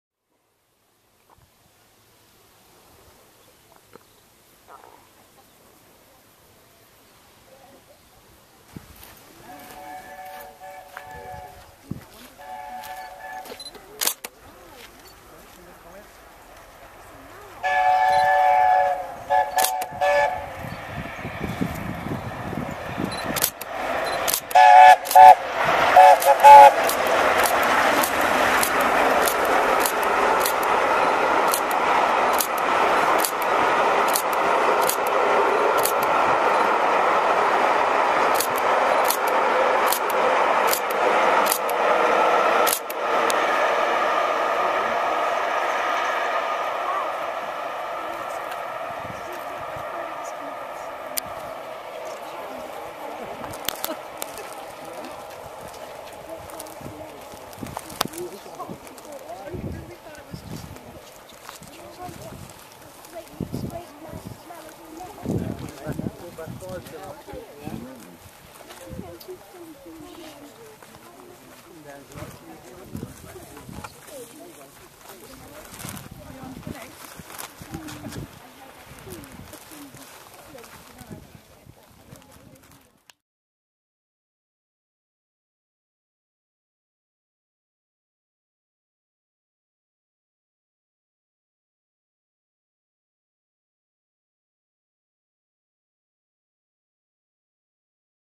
Union of South Africa Steams through Dawlish Warren
A Short video of LNER Class A4 No.60009 "Union of South Africa" steaming through Dawlish Warren.